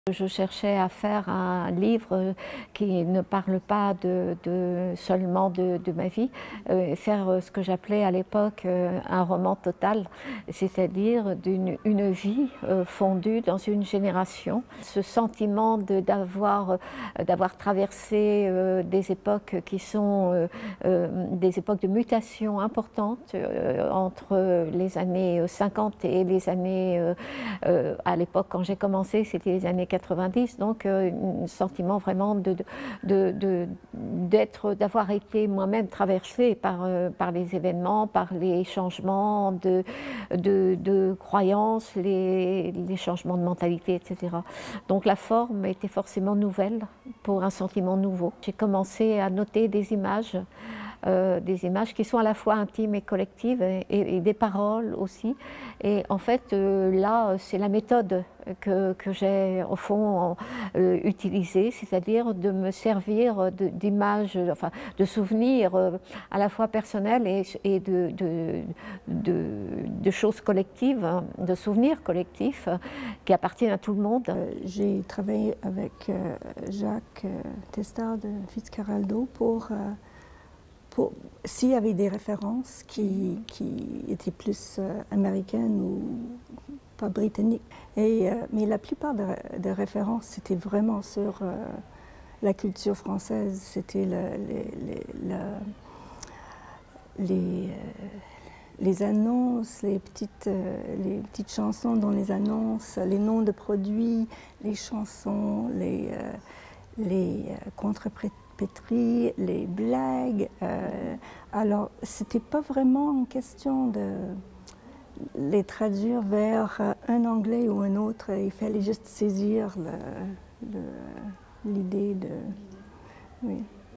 Interview Annie Ernaux & Alison Strayer
French author Annie Ernaux, awarded the 2022 Nobel Prize in Literature, was at the Institut français in 2019 with translator Alison Strayer on the occasion of the announcement of the shortlisting of her novel The Years for the Man Booker Prize.